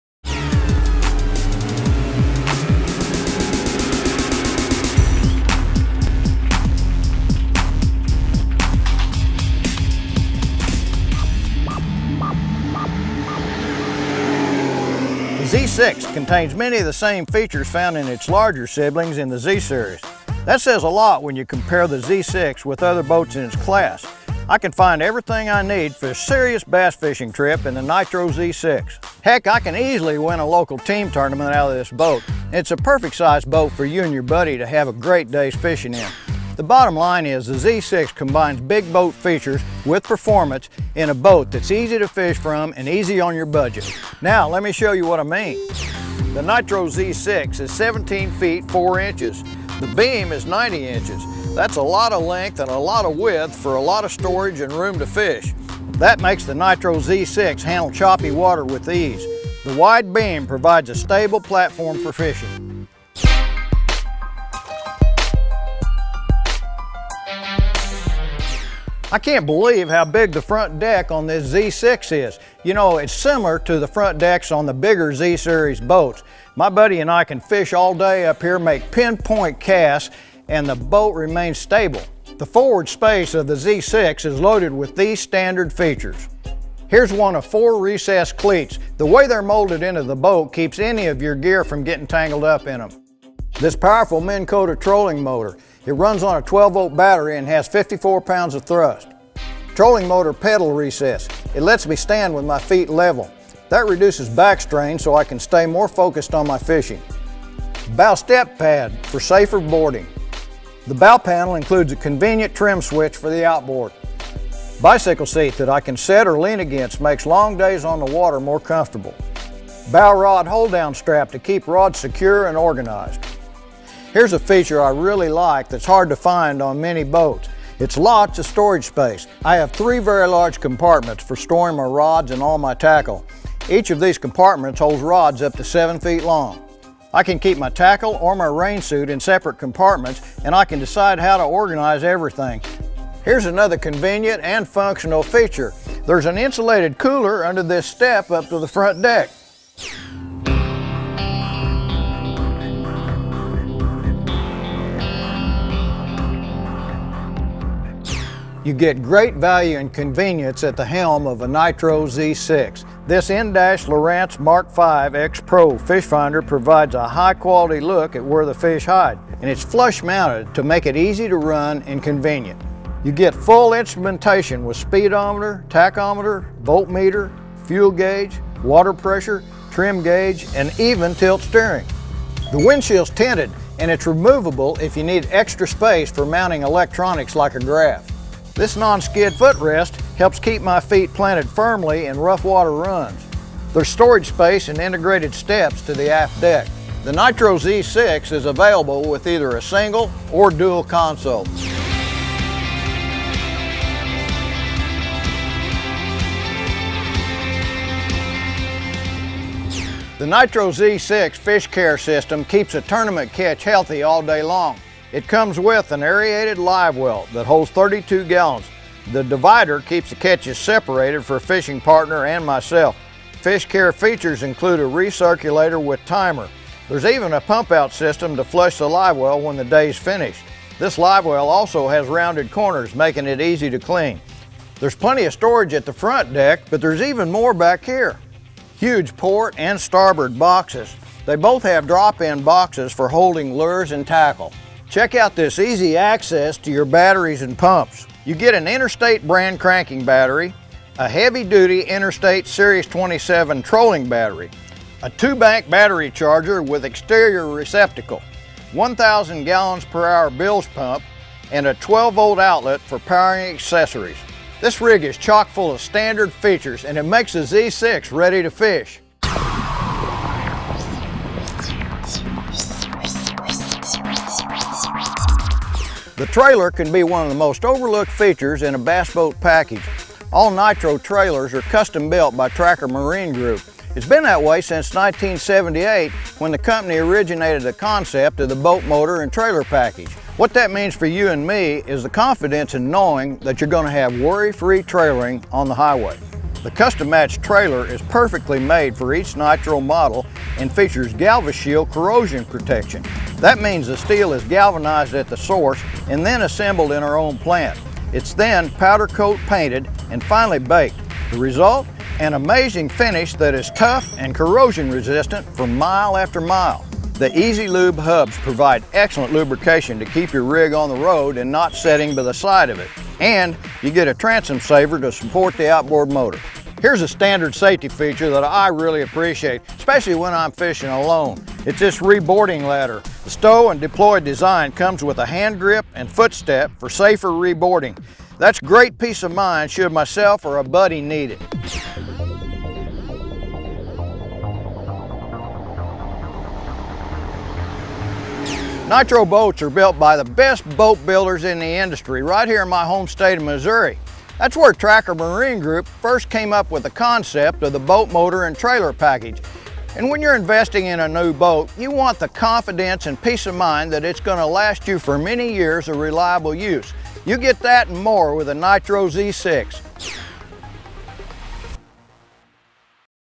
NITRO Boats: 2015 Z6 Walk Around Review with Stacey King
NITRO Pro Team Angler Stacey King gives a walkaround review of the Z6 performance bass boat.

Beam — 90"
Storage Length — 18' 10"
Maximum person capacity — 3